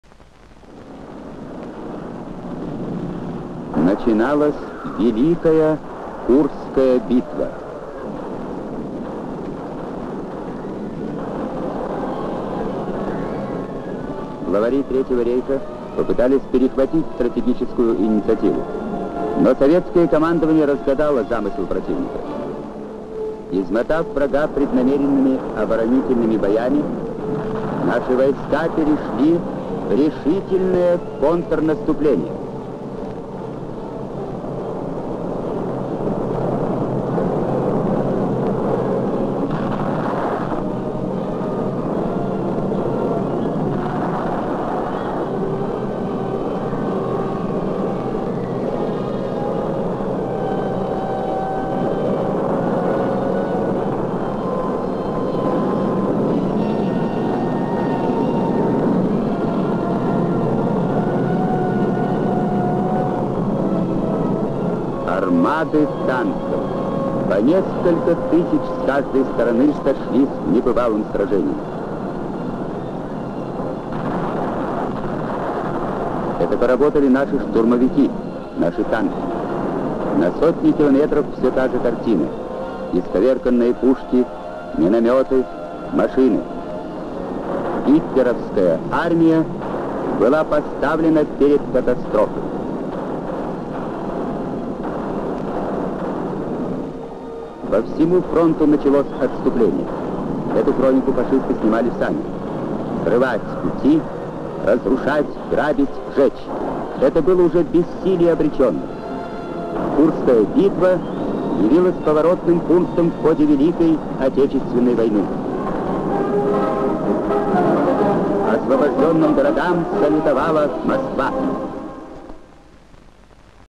КУРСКАЯ БИТВА. (июль-август 1943г.)
*История СССР на кадрах кинохроники*
_Оцифровано с 16мм фильмокопии Vladimir Malik Channel._
_Показ фильма предназначен для  информационных, научных, учебных и культурных целей._
*Спасибо большое за Вашу помощь!.*